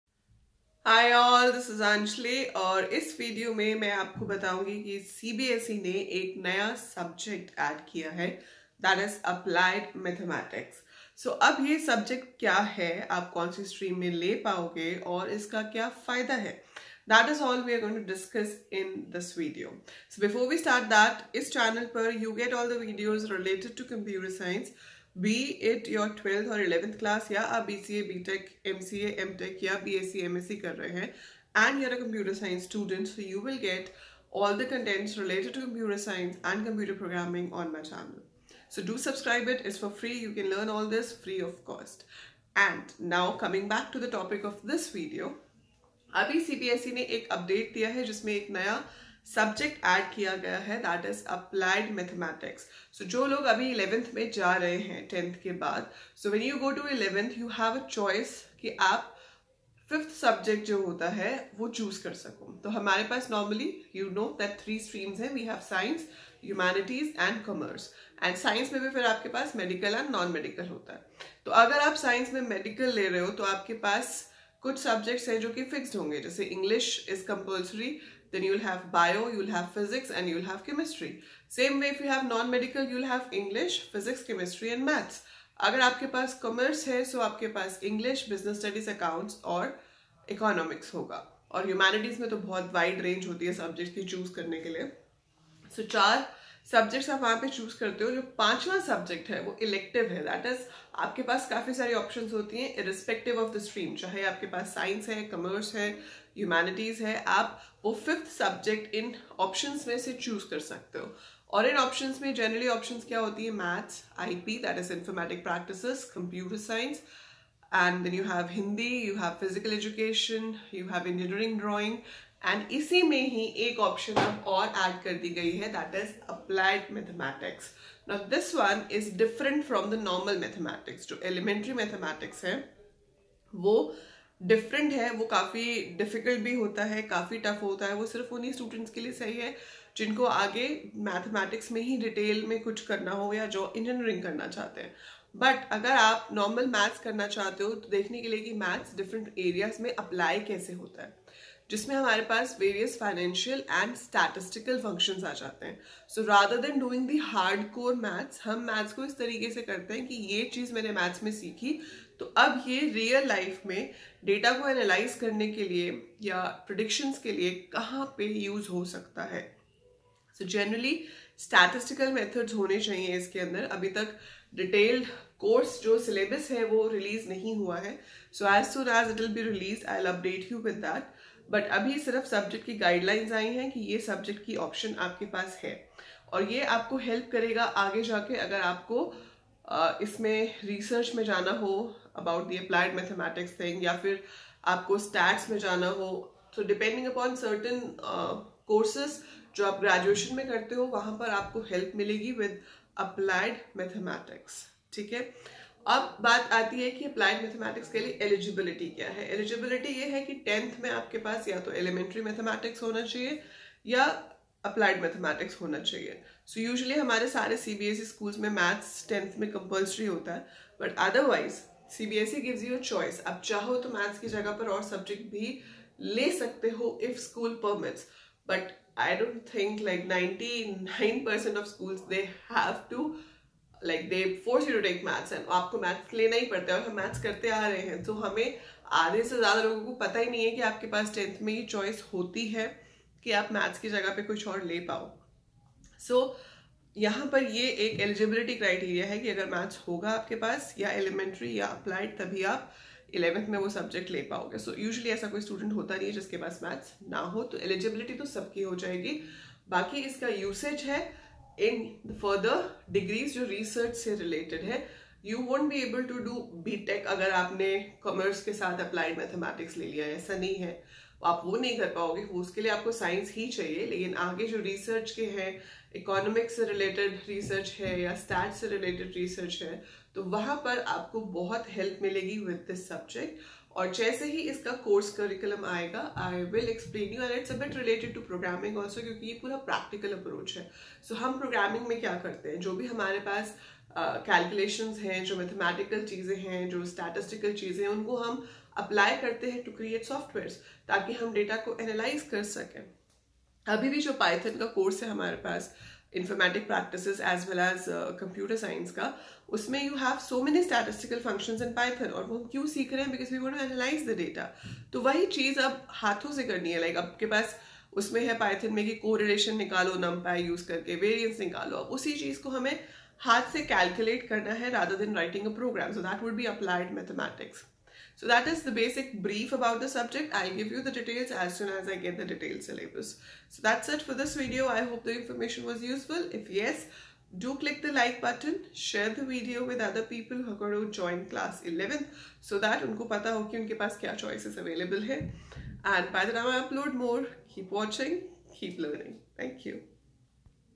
CBSE Introduces new subject Applied Mathematics for class XI (2020-21) as an elective subject
CBSE Update :- A new subject is introduced by CBSE named Applied Mathematics from year 2020 for class XI as an elective student.